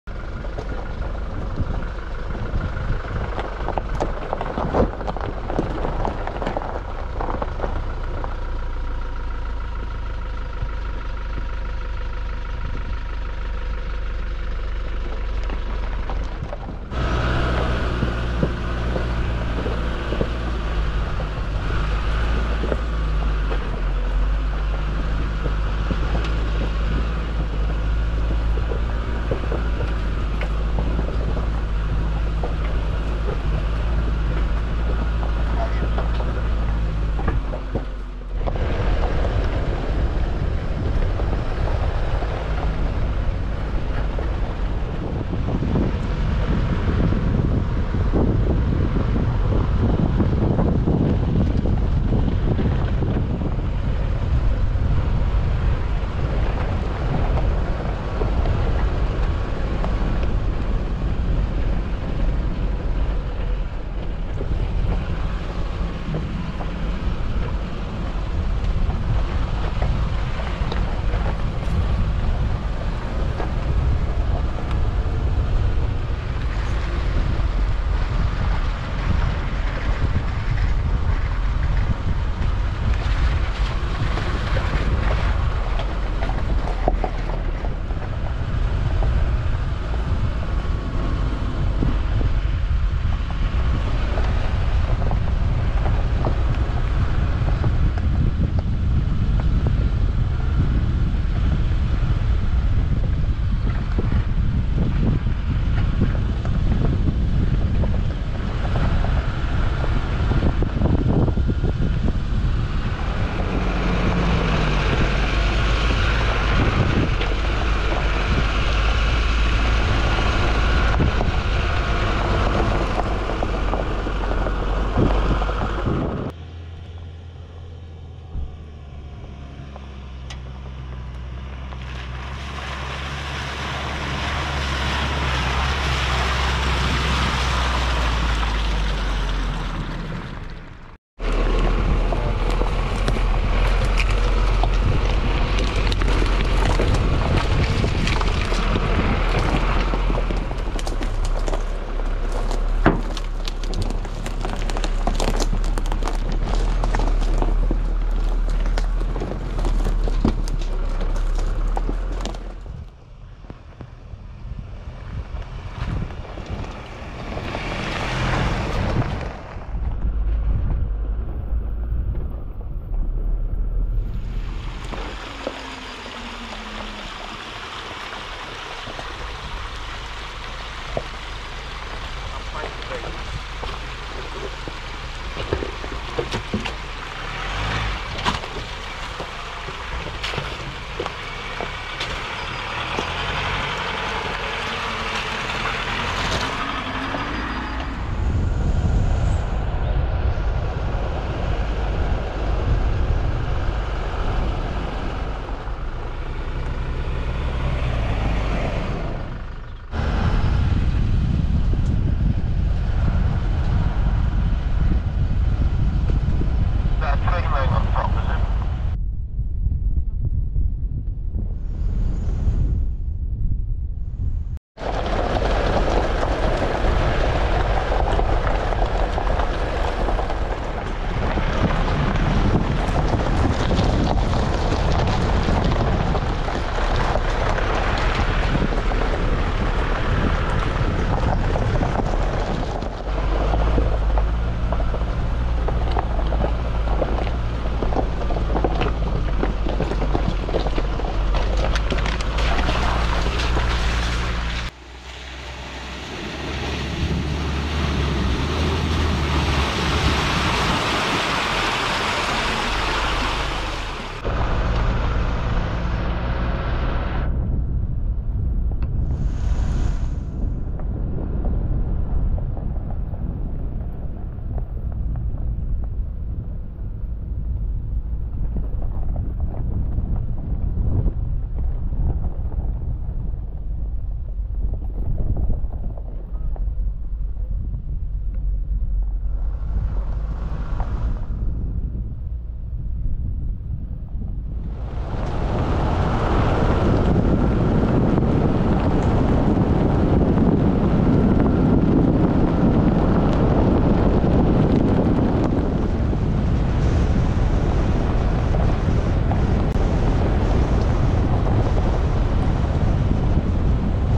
4x4 Wales post lockdown part 2
Ford Ranger Raptor and Toyota Hilux revo green laning in Carmarthenshire.